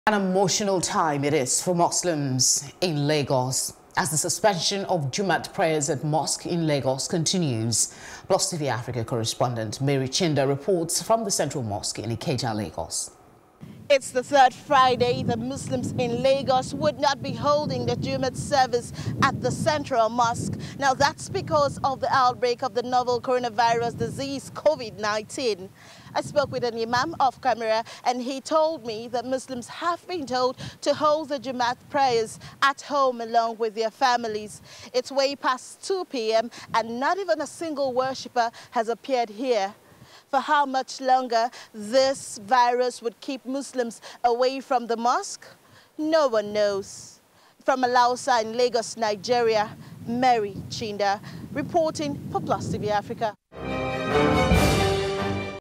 Covid-19 Lockdown: Muslims Pray From Home
An emotional time it is for muslims I'm Lagos as the suspension of Jummat Prayers at mosques in Lagos continues.